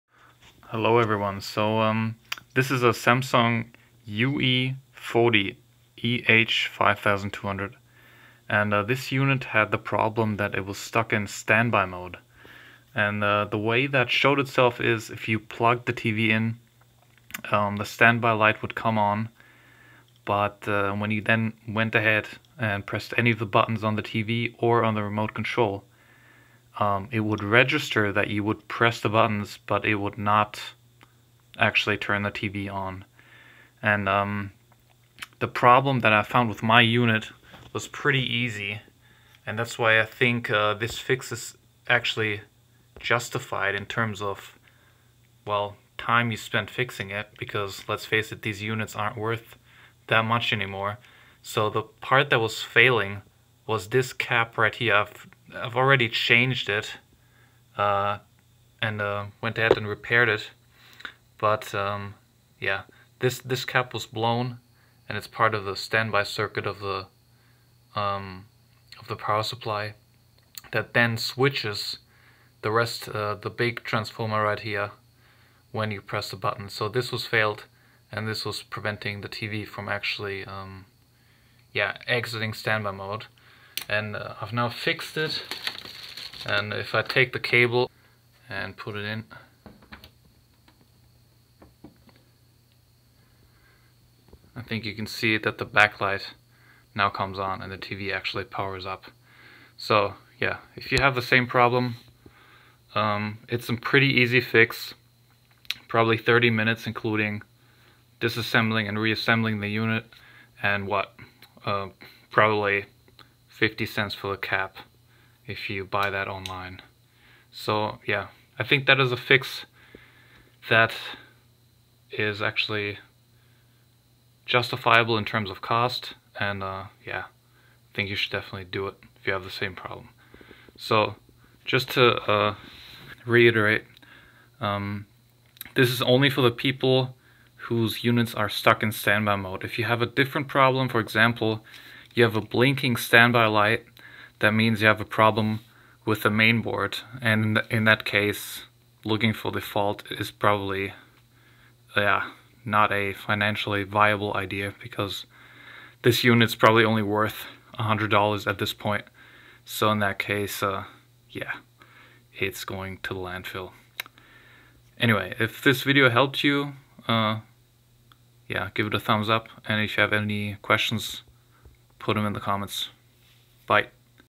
Samsung UE40EH5200 stuck in standby - 0,50$ FIX - | BN44-00498A Power Supply Board | bad cap on psu
Is your Samsung TV stuck in standby mode? This might be the solution.

The fix is easy, replacing the blown capacitor on the psu - standby circuit.

original cap: SamYoung 10v 1200uF
replace cap: Panasonic 16v 1200uF

Just replace the cap with a better brand with the same or better values. Height of cap should not exceed 30mm.